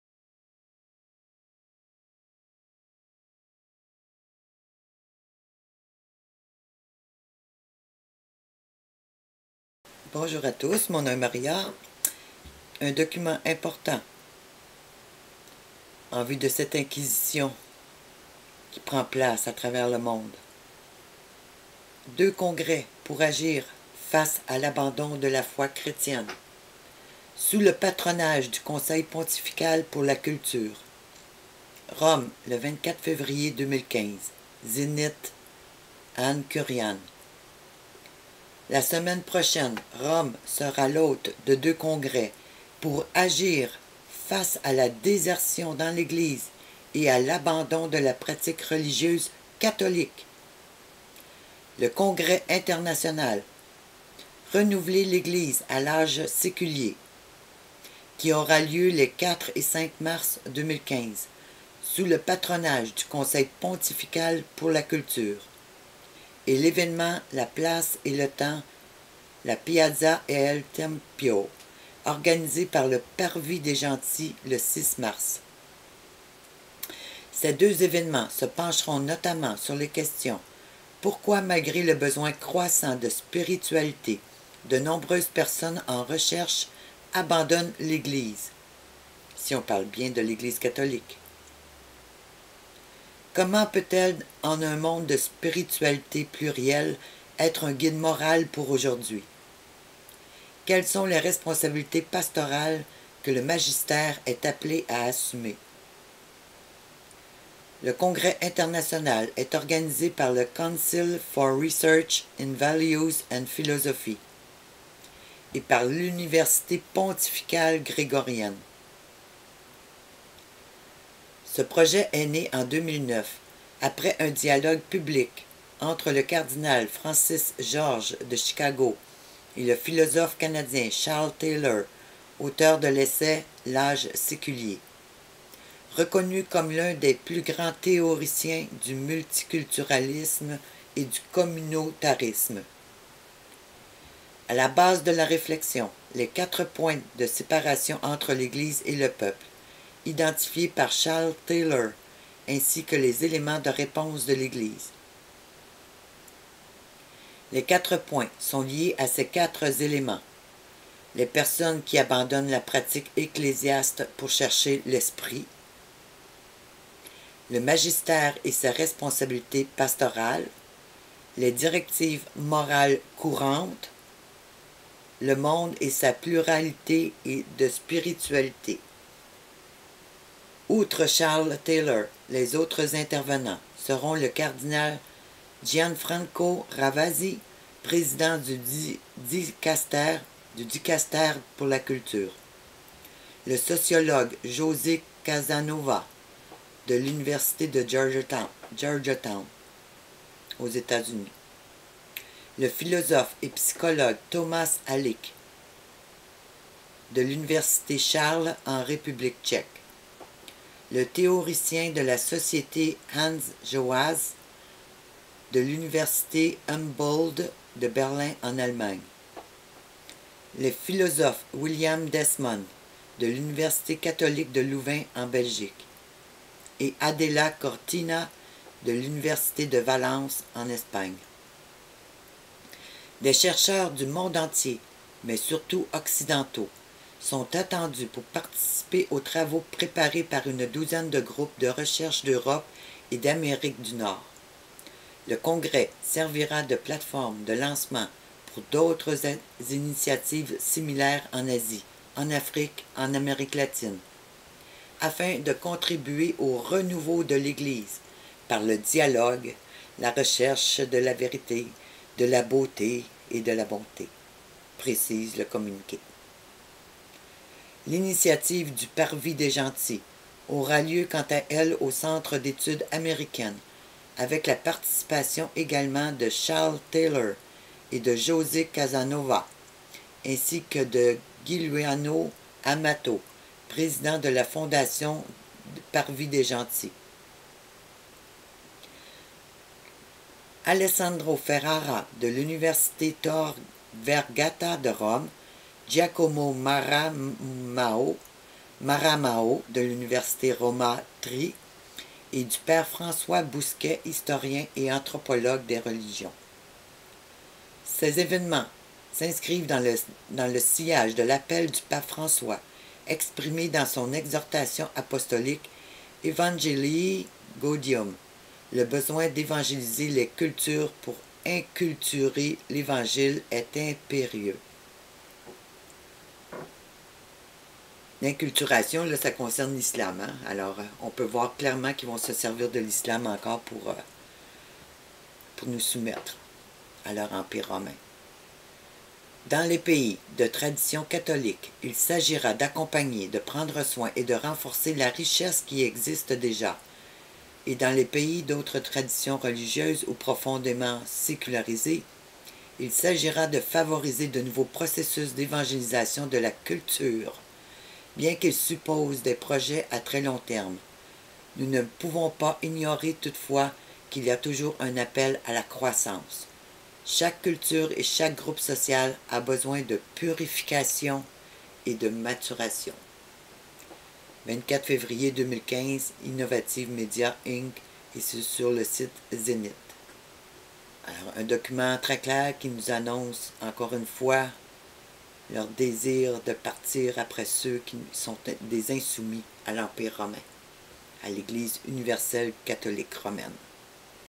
INQUISITION DEUX CONGRES POUR AGIR FACE A L'ABANDON DE LA FOI CHRETIENNE
DEUX CONGRES POUR AGIR FACE A L'ABANDON DE LA FOI CHRETIENNE
Conseil Pontifical de la Culture
4-5 Mars 2015